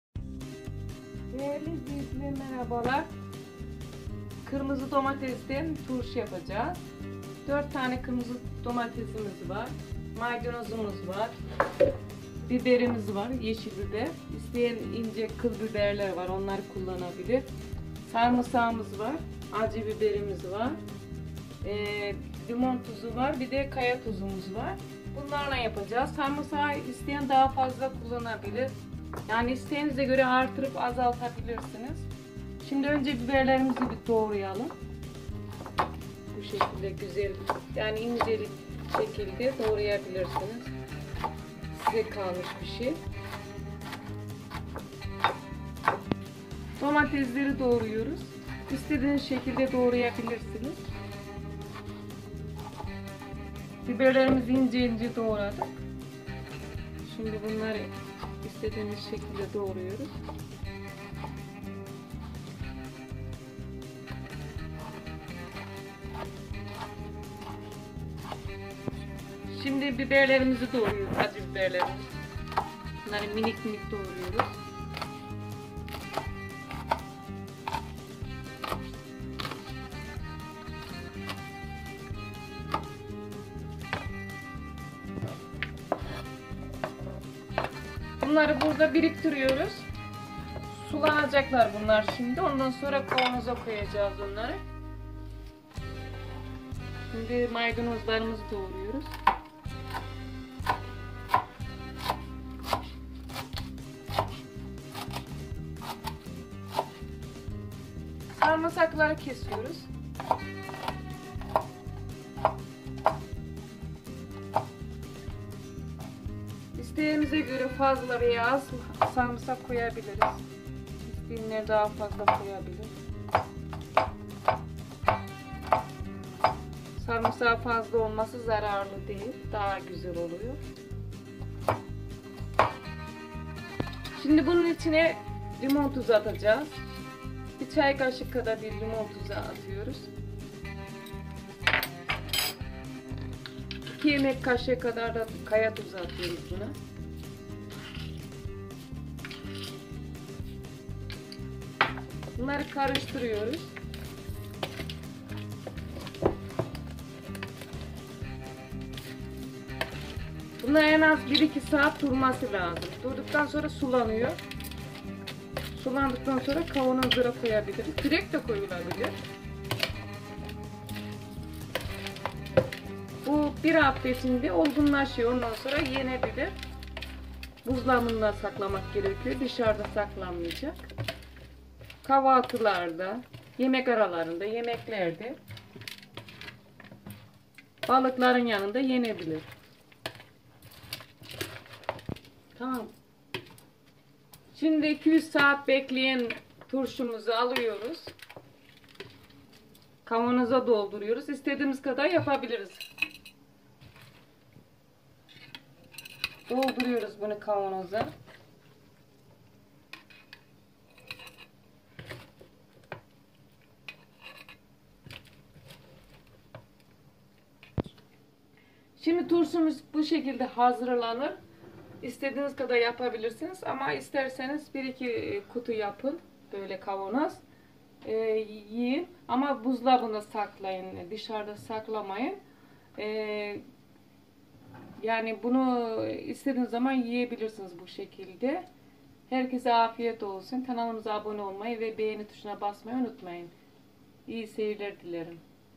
KAHVALTIILIK KIRMIZI DOMATES TURŞUSU(ÇOK KOLAY, LEZZETLİ ,KAHVALTI İÇİN)TURŞU
Malzemeleri
4 Domates
4-5 sivri biber ya da kıl biber, kııl biberle daha güzel oluyor.
5-6 sarımsak fazla da konabilir.
2-4 acı biber az da olabilir.
Yarım demet maydanoz
1 tatlı kaşığı limon tuzu
2 kaşık kayatuzu
Hepsini  önce ayıklayıp yıkarız.küçük doğranır.akşam dan istenen zamanda bir kaba konur.tuzu ve limon tuzu da eklenir.Tuz miktarı kendi damak tadınıza göre ayarlanır. Kesinlikle su eklenmez.Kendi suyu yeterlidir. Tüm suyuyla  kavanozlara konur.Biberleri sararınca yenmeye hazırdır.  Bir iki gün içinde bile yemeye başlayabilirsiniz. Buzdolabında saklayarak tüketebilirsiniz.salata yerine de kullanılabilir siniz.

İyi seyirler..